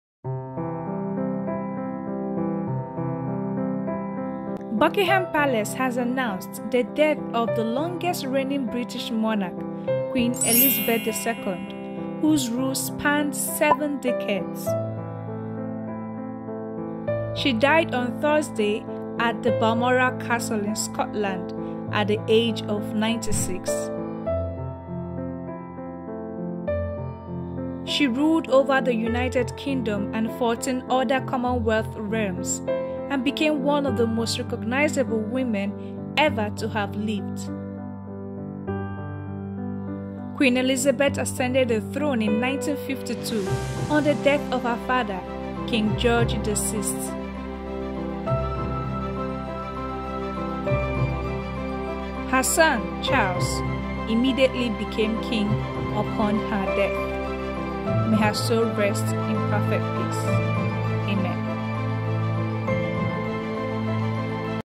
Breaking News: Queen Elizabeth II Dies At Age 96
Queen Elizabeth II died on Thursday, September 8, 2022 at the age of 96 at Balmoral palace in Scotland.
May her soul rest in peace, Amen!